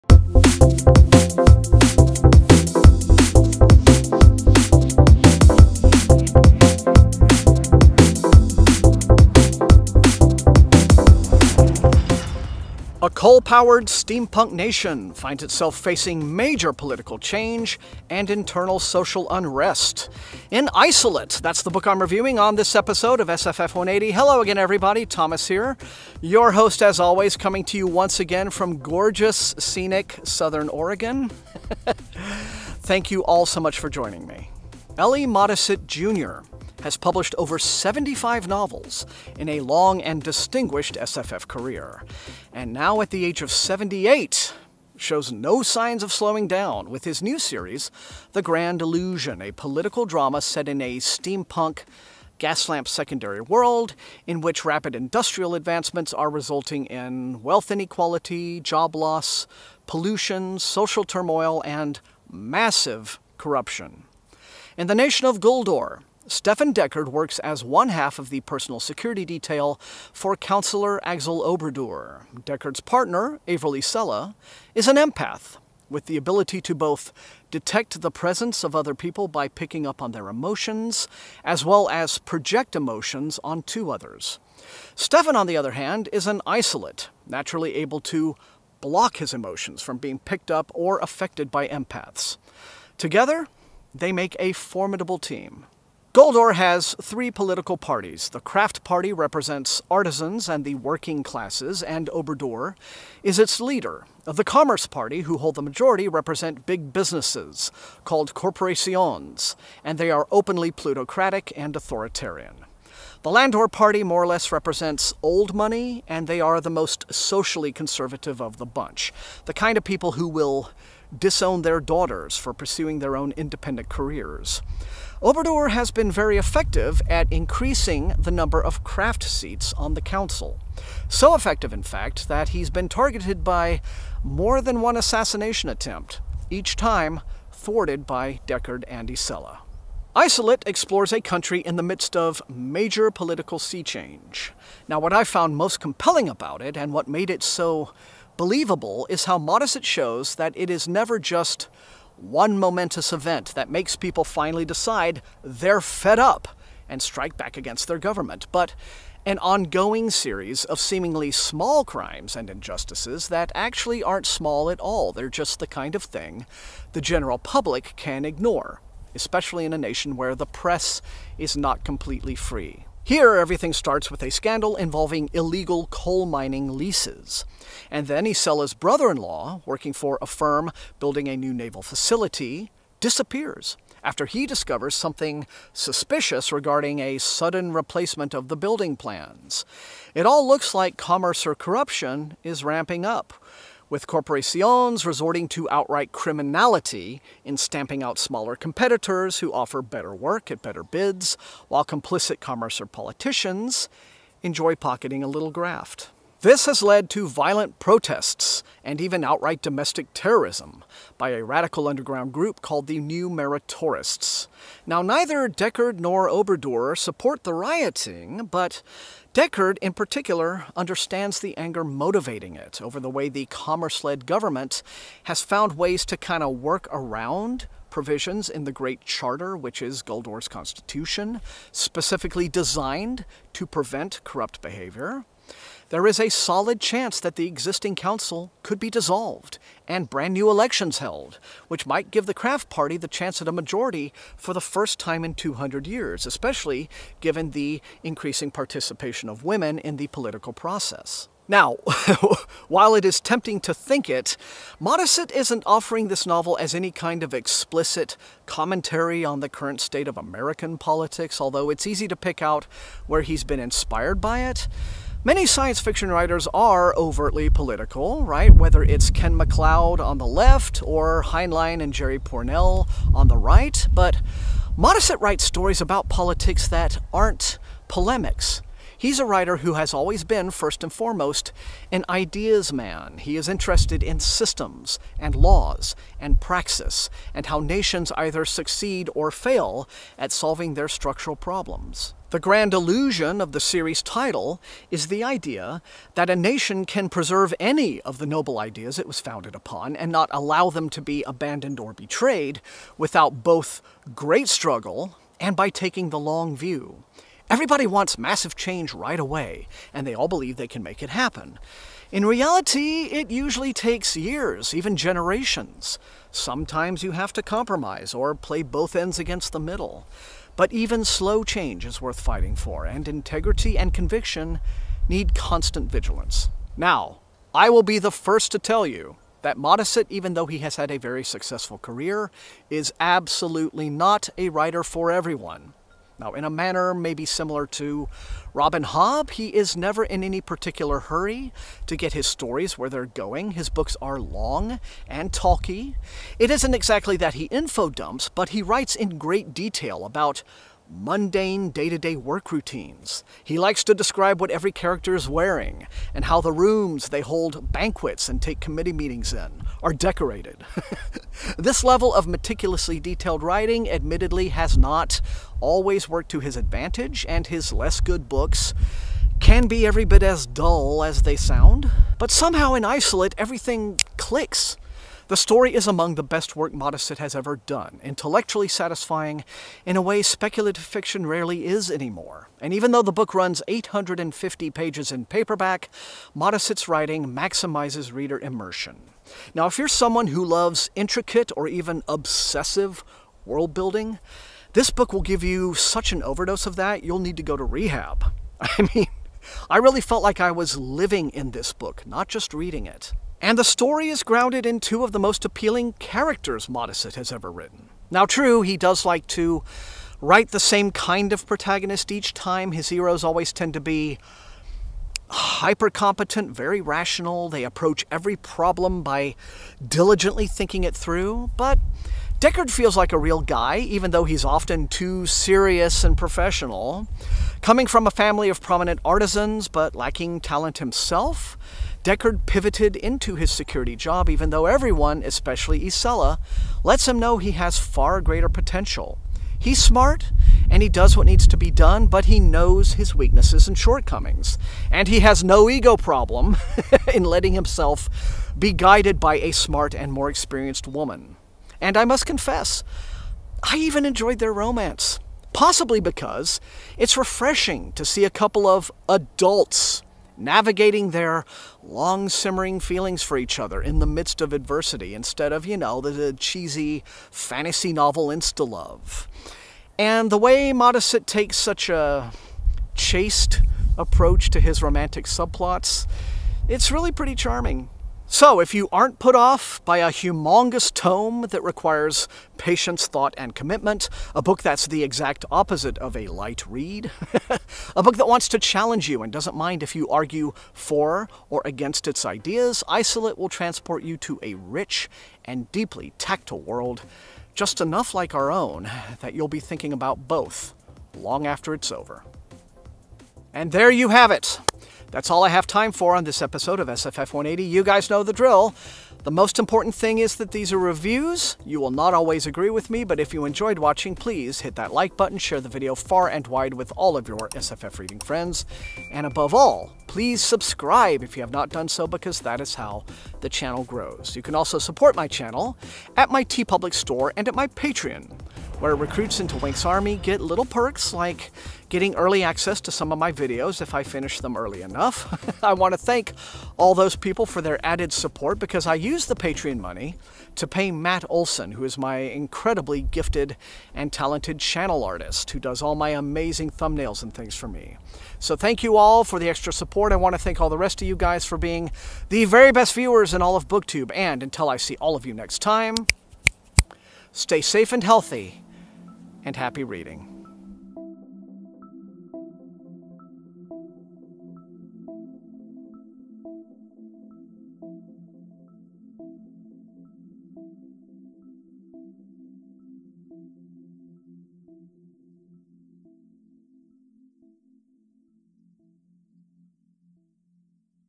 SFF180 🏛 ‘Isolate’ by L.E. Modesitt, Jr. ★★★★
A coal-driven steampunk nation faces major political change and social unrest, as two security agents try to protect a party leader working to expose corrupt rivals.

Support SFF180!